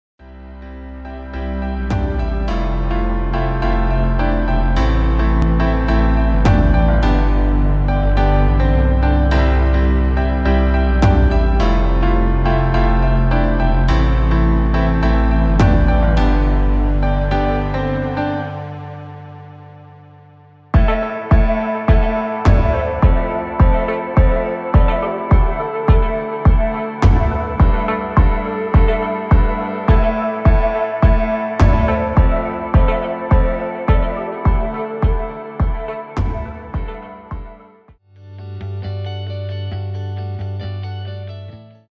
PATIENT SPOTLIGHT: Right Transfemoral Amputee
After just three weeks, practicing walking with his right transfemoral prosthesis and he is rocking it. We just need to work on his posture, which I am sure he gets sick and tired of me saying! He has been an awesome guy to work with and am so proud of his hard work. Keep it up man!